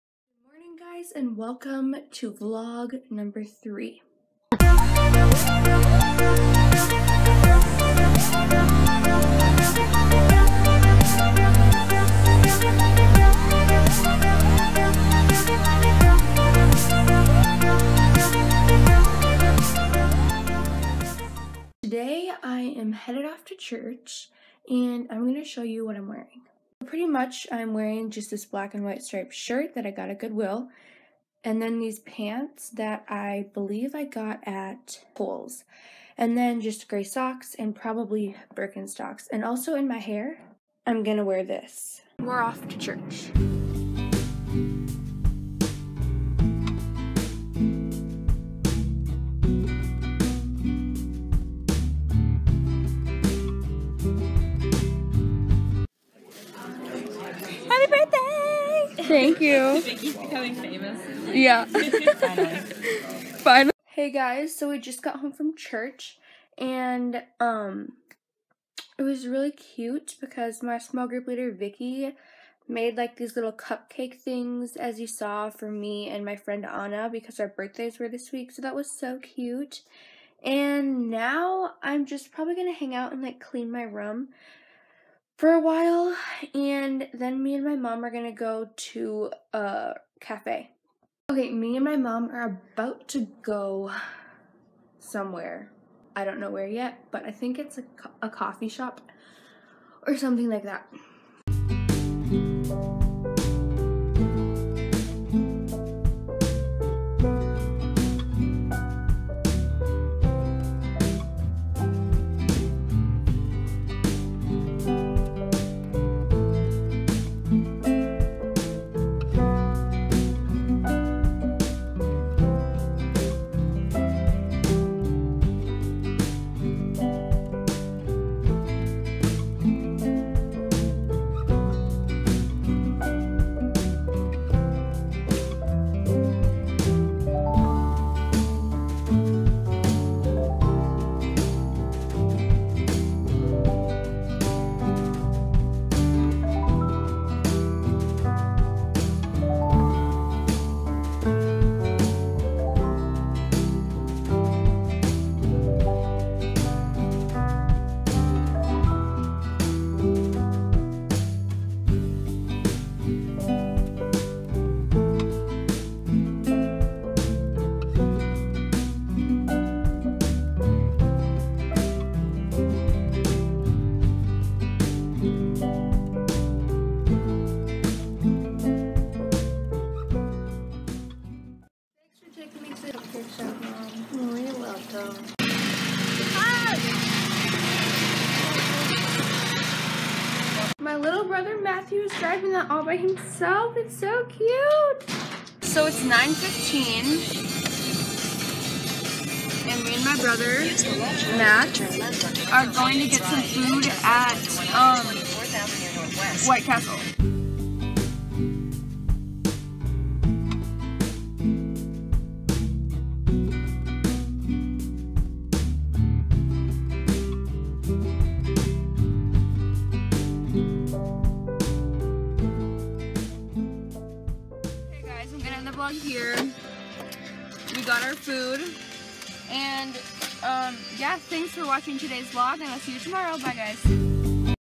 THE CUTEST CUPCAKE SHOP!!
TODAY ME AND MY MOM ATE AT THE CUTEST CUPCAKE SHOP